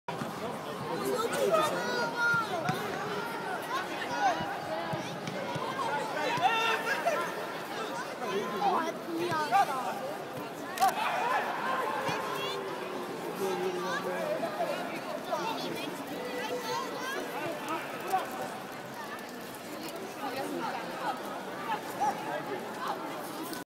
11/10/25: PSV traint in Philips Stadion
PSV trainde dinsdagochtend in het Philips Stadion.